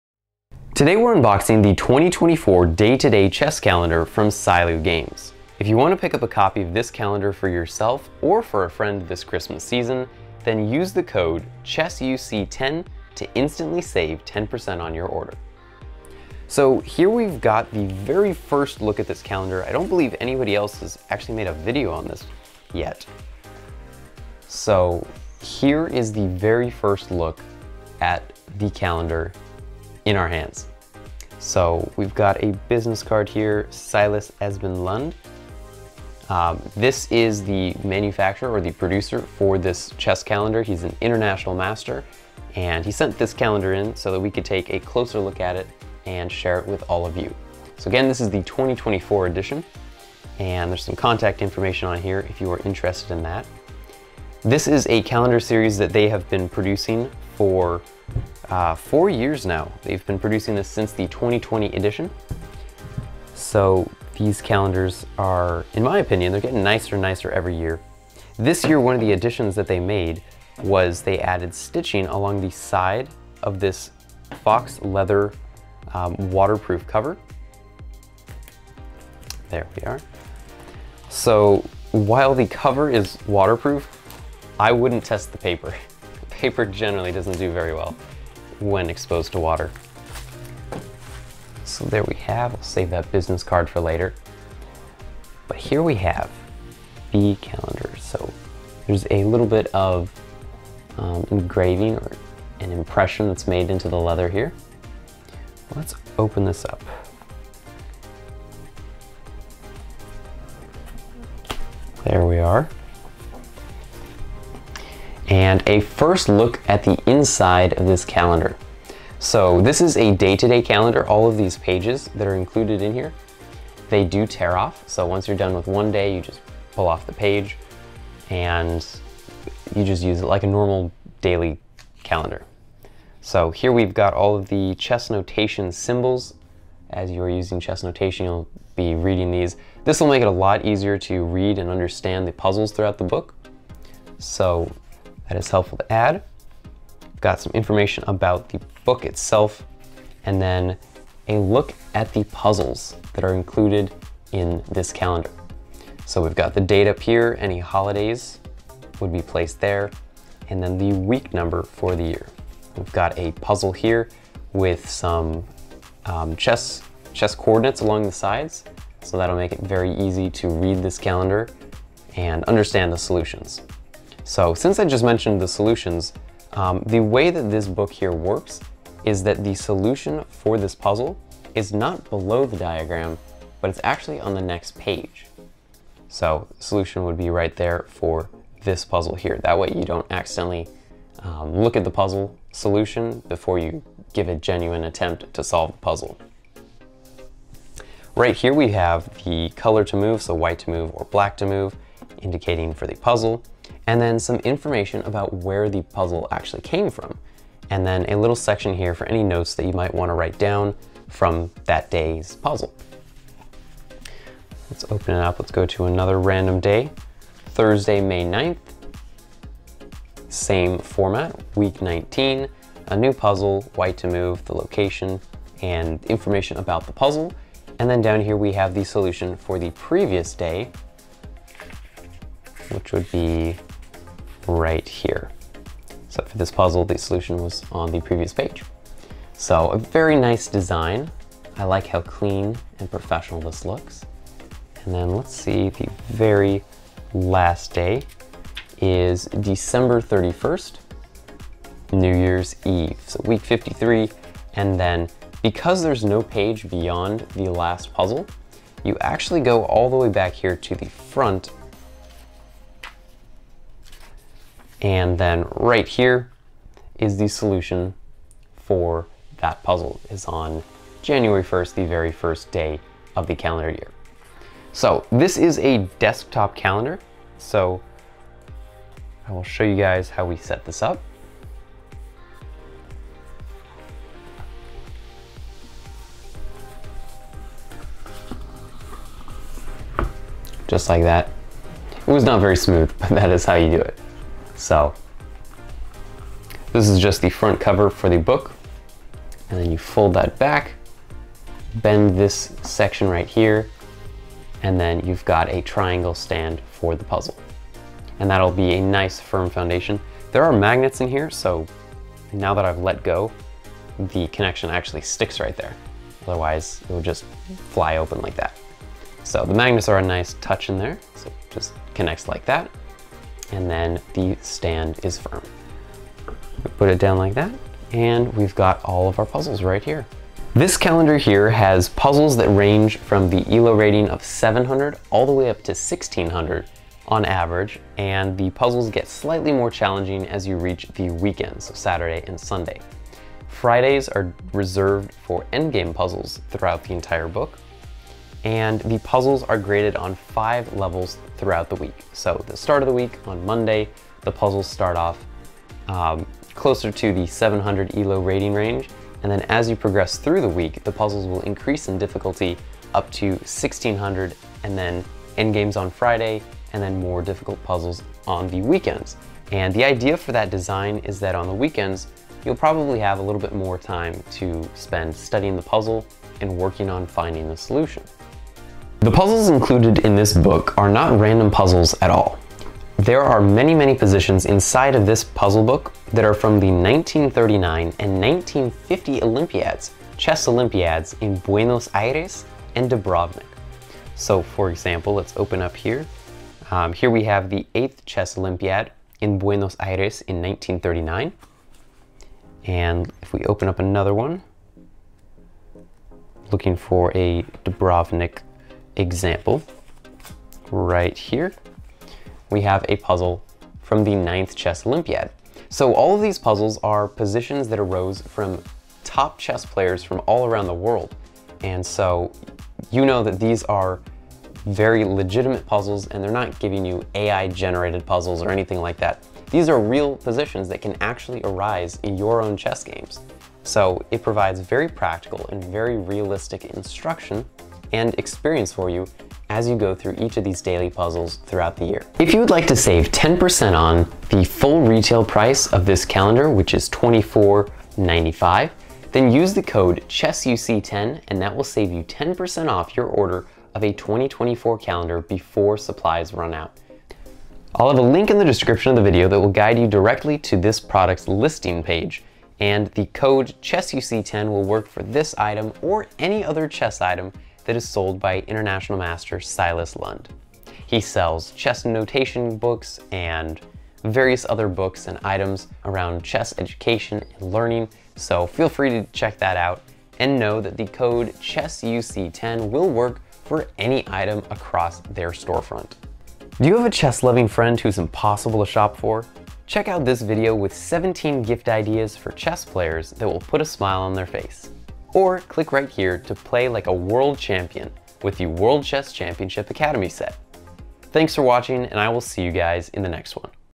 Unboxing the 2024 Day-to-Day Chess Calendar | A Year of Chess Puzzles
✉️ Get the insider's scoop on all things chess - delivered to your inbox

~ Timestamps ~

0:00​ Revealing the New 2024 Chess Calendar
1:00 Unboxing the Calendar
1:51 A Look at the Puzzles
4:54 The Calendar Stand
5:30 Puzzle Details
7:23 10% Off Discount Code!
7:43 About the Manufacturer
8:06 Watch Next

Thanks to  @silasesbenlund6941  for supplying the chess calendar seen in this video!